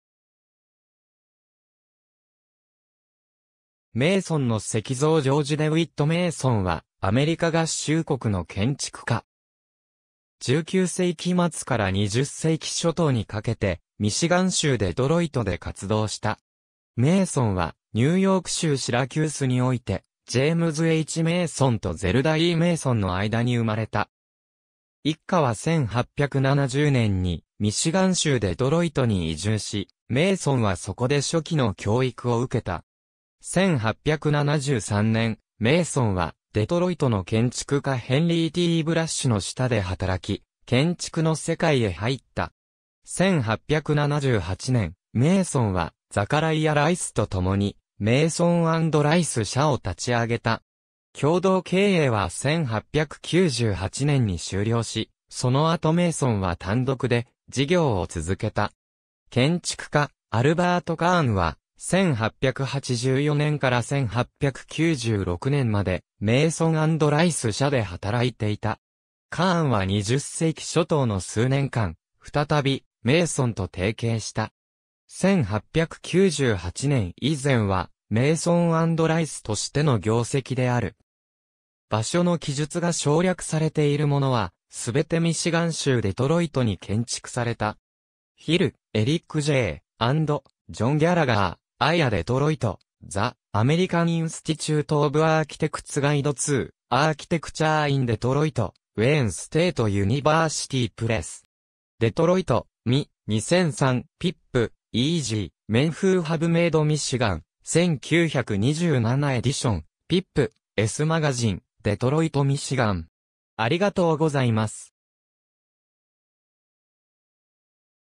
ジョージ・デウィット・メイソン
メイソンの石像 ジョージ・デウィット・メイソン（1856年 - 1948年）は、アメリカ合衆国の建築家。
19世紀末から20世紀初頭にかけてミシガン州デトロイトで活動した。
メイソンはニューヨーク州シラキュースにおいて、ジェイムズ・H・メイソン (James H. Mason) とゼルダ・E・メイソン (Zelda E. Mason) の間に生まれた。
一家は1870年にミシガン州デトロイトに移住し、メイソンはそこで初期の教育を受けた。
1873年、メイソンはデトロイトの建築家ヘンリー・T・ブラッシュ (Henry T. Brush) の下で働き、建築の世界へ入った。
1878年、メイソンはザカライヤ・ライス (Zachariah Rice) とともに「メイソン・アンド・ライス」社を立ち上げた。
共同経営は1898年に終了し、その後メイソンは単独で事業を続けた。
建築家アルバート・カーンは、1884年から1896年まで「メイソン・アンド・ライス」社で働いていた。
カーンは20世紀初頭の数年間、再びメイソンと提携した。
1898年以前は、「メイソン・アンド・ライス」としての業績である。
場所の記述が省略されているものは、すべてミシガン州デトロイトに建築された。
^ Hill, Eric J., and John Gallagher, AIA Detroit: The American Institute of Architects Guide to Architecture in Detroit, Wayne State University Press,
Detroit, MI, 2003. ^ Pipp, E.G., Men Who Have Made Michigan, 1927 Edition, Pipp's Magazine, Detroit Michigan.